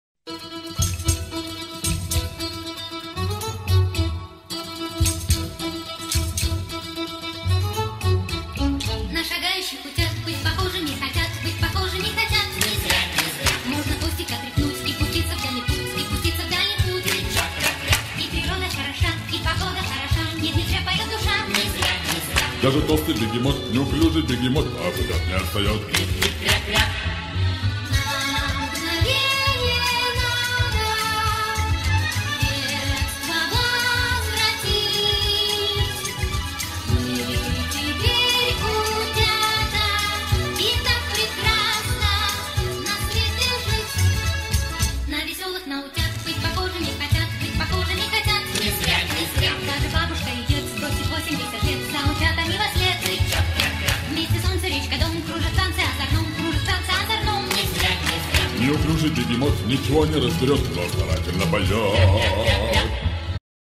Танцующий малыш - 9 месяцев - Dancing 9 month old Dylan
Дилан растанцевался вместе с дедушкой на вечеринке у друзей в честь дня благодарения.
Dylan had fun at Thanksgiving party with his grandfather.